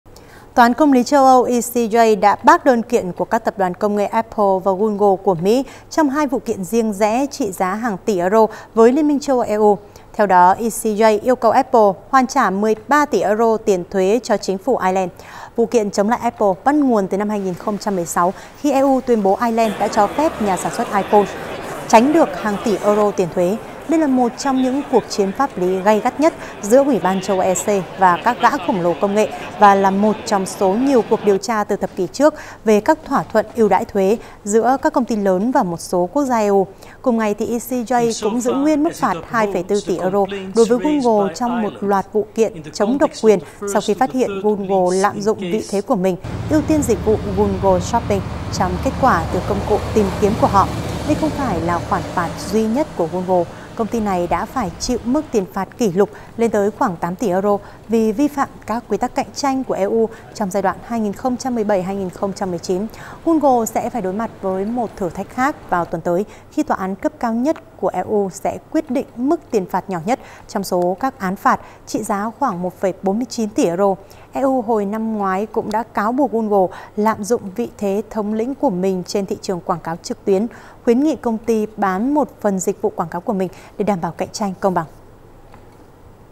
EU thắng kiện các tập đoàn công nghệ Mỹ | Tin tức
Tòa án Công lý châu Âu (ECJ) đã bác đơn kiện của các tập đoàn công nghệ Apple và Google của Mỹ trong hai vụ kiện riêng rẽ trị giá hàng tỷ euro với Liên minh châu Âu (EU). Theo đó, ECJ yêu cầu Apple hoàn trả 13 tỷ euro tiền thuế cho Chính phủ Ireland